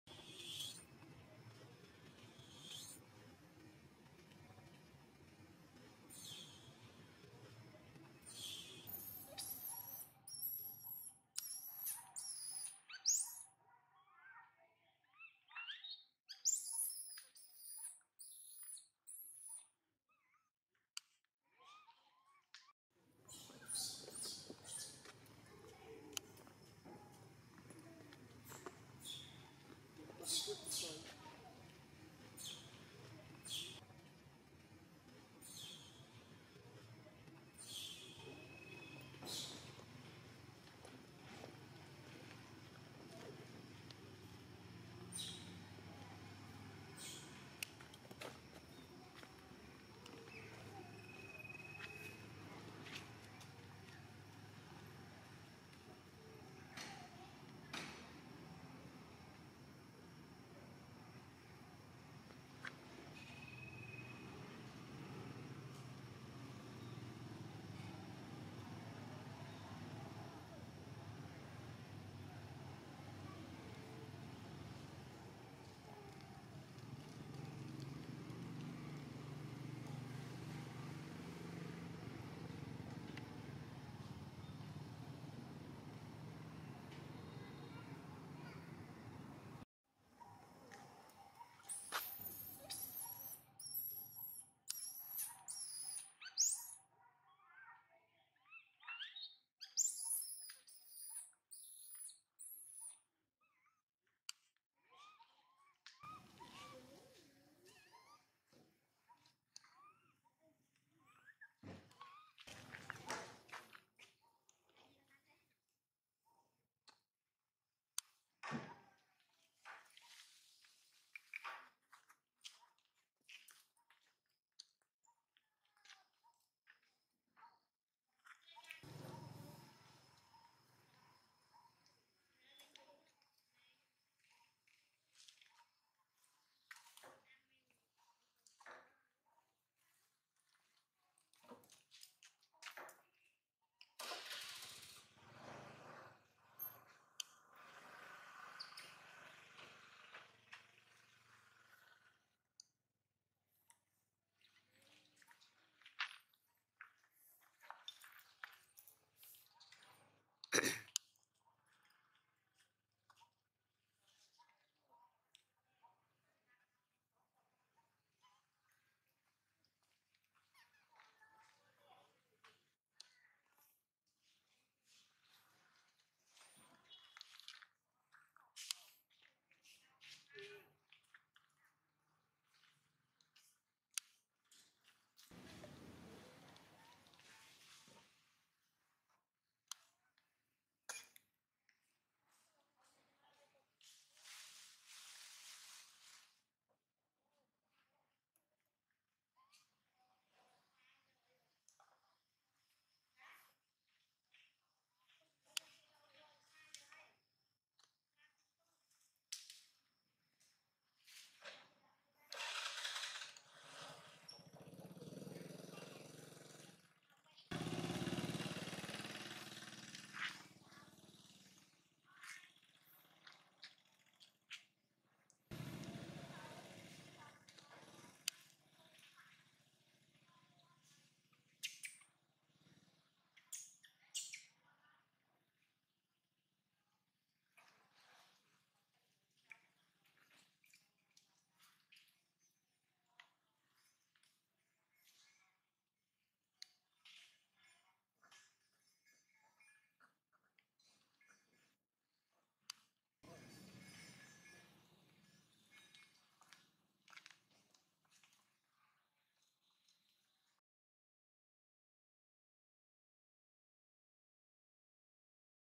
Poor Baby Emilo Is Woke Up By Strongly Warning Of Emila Mother
Welcome to channel Monkey Releaser. Today, I would like to share you the video of Poor Baby Emilo Is Woke Up By Strongly Warning Of Emila Mother. In this video is showing the activity of poor mother Emila wakes her beloved baby Emilo up by strongly warning till he runs out to other for help.